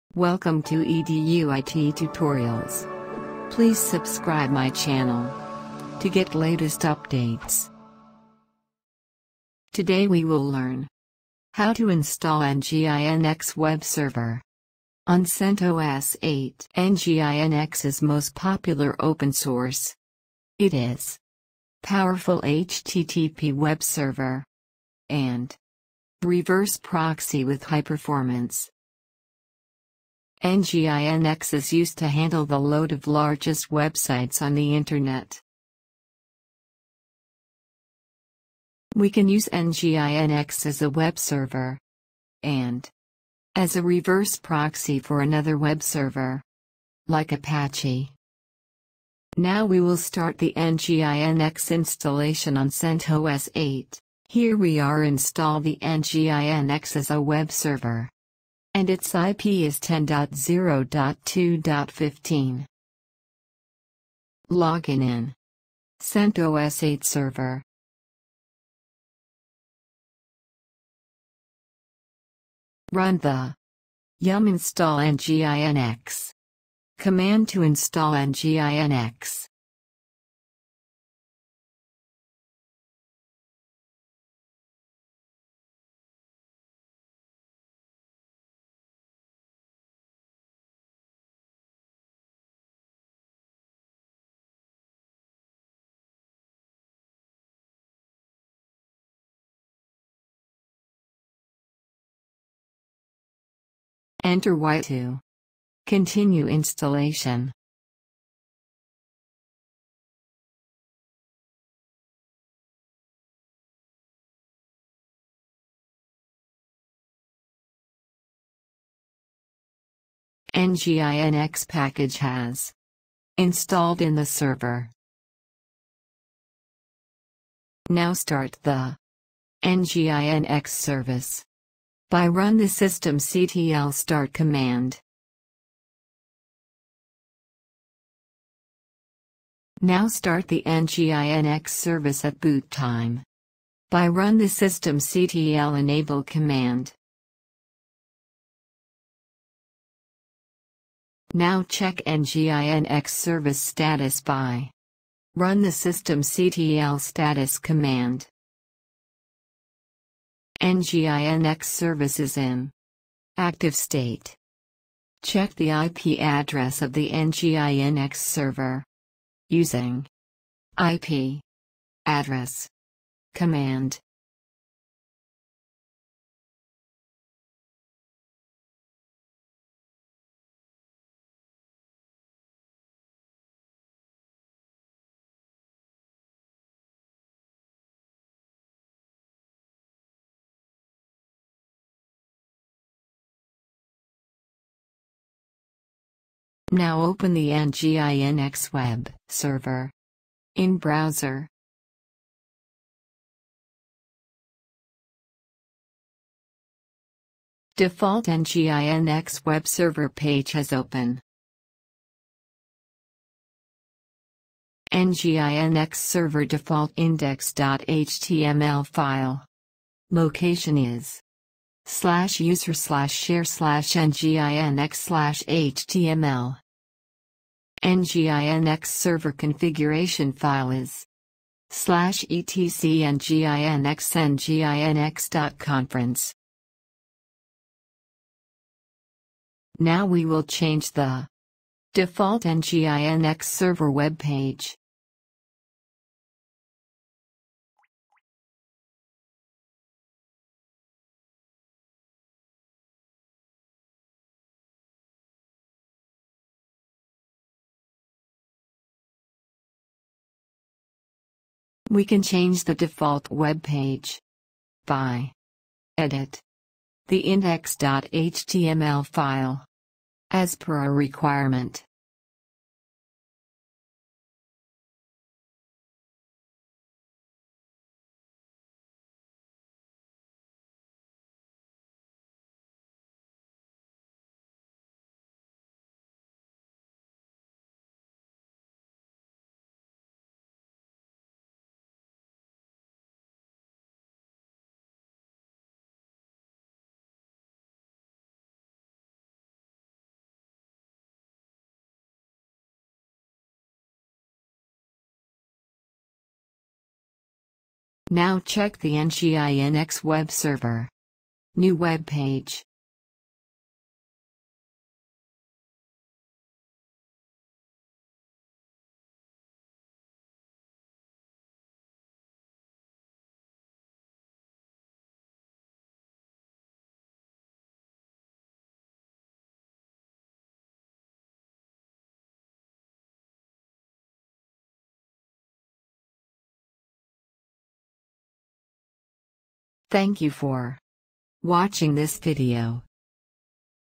Linux Tutorial | Nginx web server installation in CentOS8 or RHEL8
In this video show how to configure nginx as a webserver in Centos 8,welcome to nginx on red hat enterprise linux,